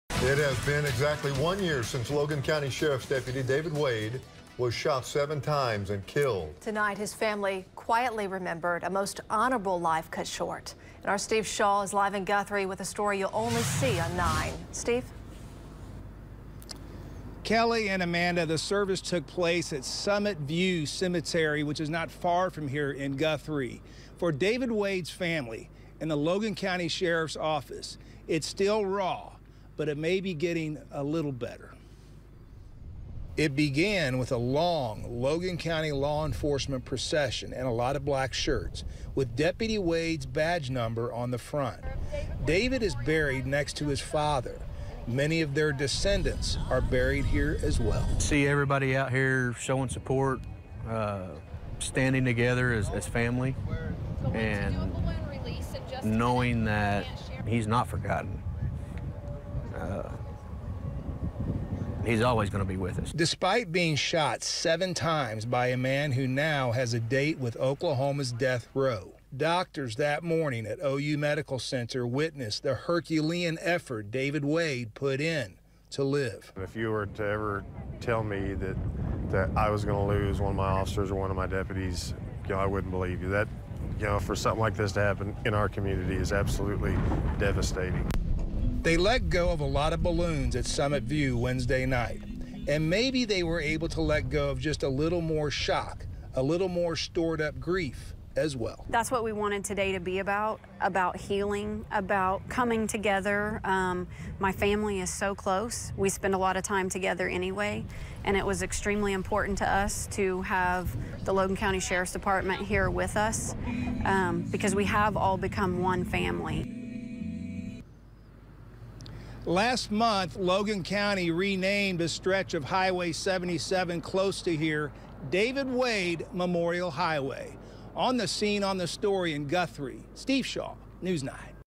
Family, Community Honors Fallen Deputy On 1 Year Anniversary
Wednesday was the one-year anniversary of the gunshot murder of Logan County Sheriff&rsquo;s Deputy David Wade.&nbsp;&nbsp;